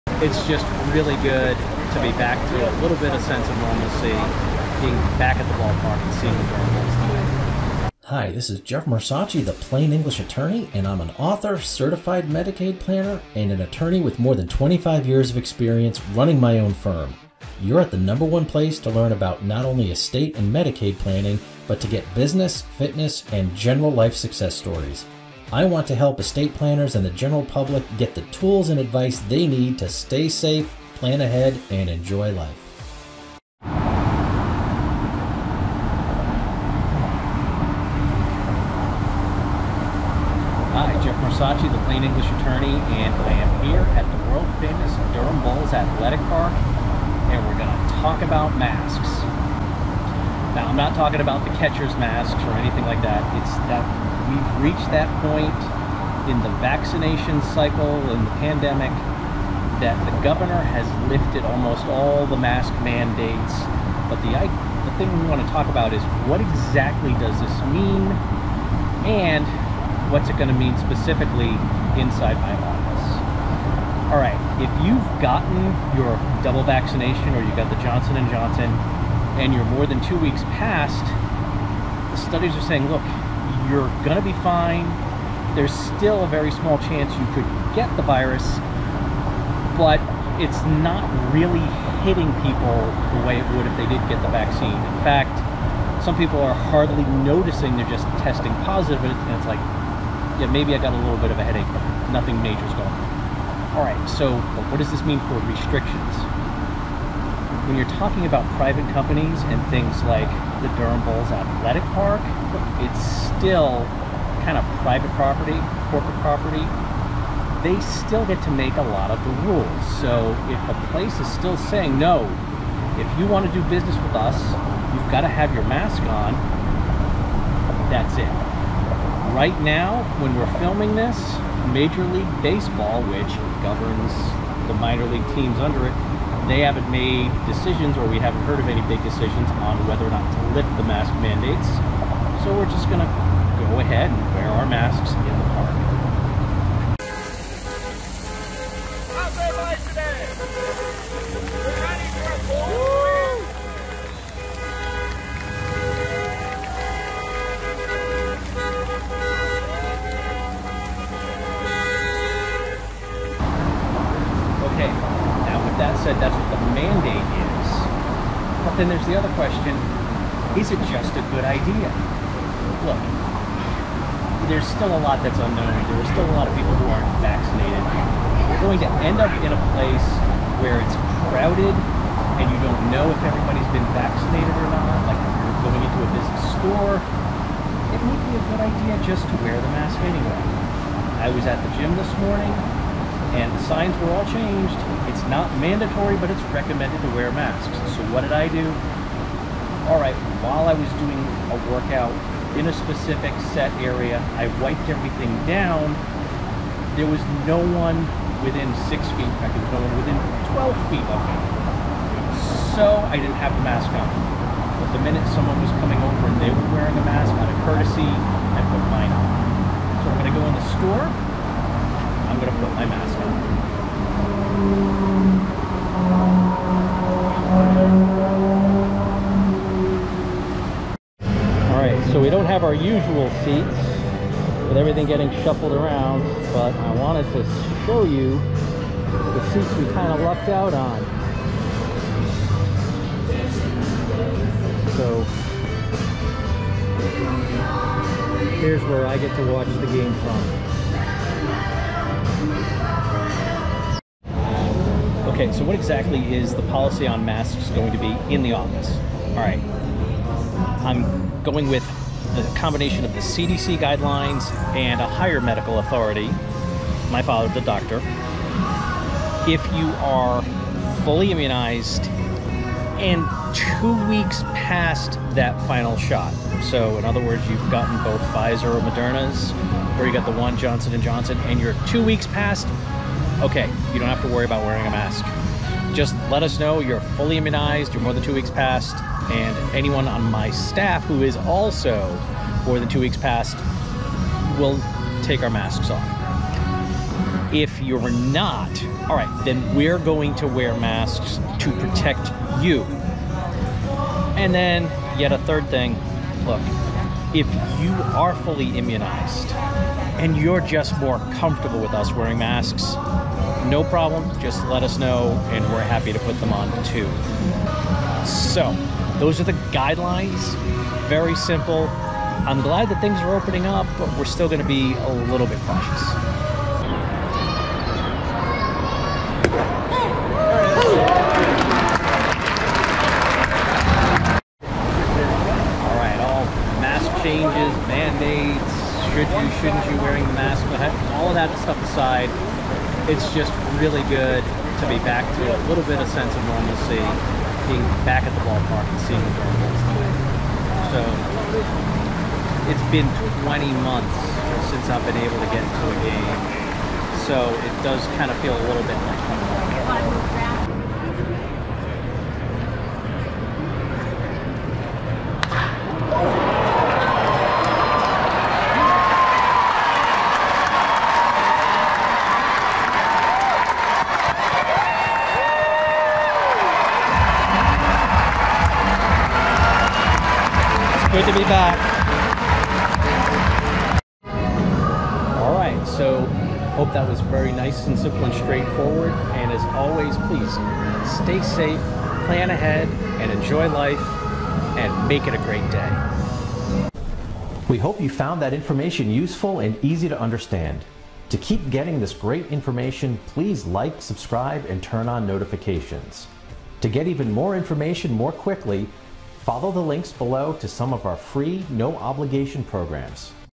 A Grand Slam or a S-icide Squeeze Play?
It was a great homecoming to get to a Durham Bulls game after minor league baseball shut down for 20 months, and it happened to coincide with the CDC mask recommendations changing. But are these changes a Grand Slam in fighting the pandemic, or a S-icide Squeeze Play where others may suffer if the vaccinated stop wearing masks? And what are the new rules for the law office?